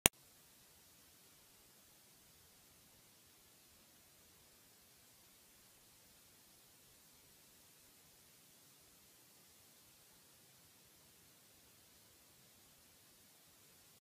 Real human temporal bone in glass dome
Real human temporal bone mounted within a glass dome- only at Memento Mori.